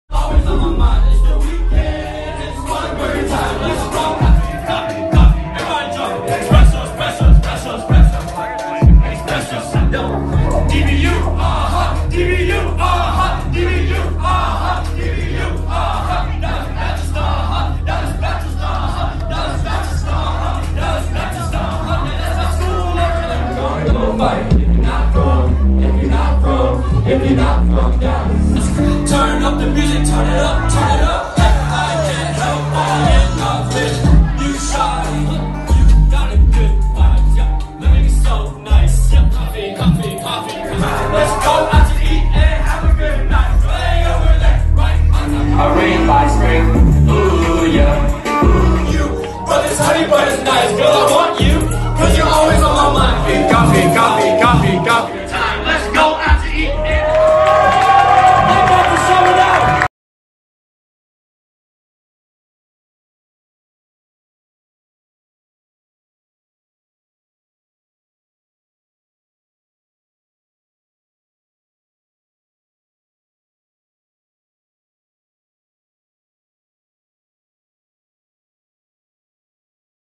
Shi-Fi - MY FIRST CONCERT (INSTAGRAM STORY VIDEOS)
My performance of DBU through all the Instagram videos my friends took! This was a concert I had at Dallas Baptist University on April 30, 2019!